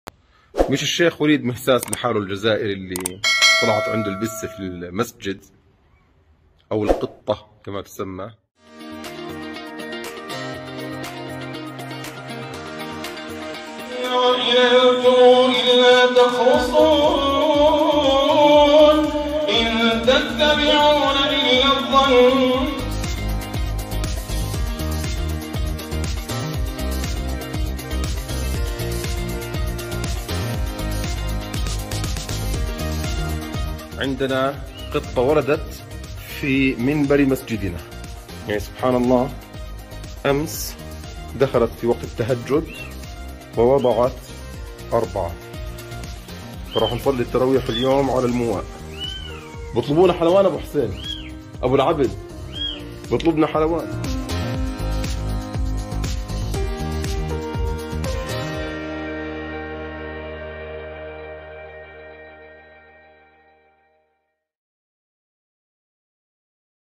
🔺ادامه داستان امام جماعت و گربه؛ این بار زایمان یک گربه در منبر مسجد
🔹شیخ معاذ عودات امام جماعت مسجدی در اردن با انتشار ویدئویی از زایمان یک گربه در مسجدی که در آن نماز جماعت اقامه می‌کند گفت: دیروز وقتی برای اقامه نماز وارد مسجد شدم گربه‌ای را دیدم که در منبر مسجد چهار قلو زایمان کرده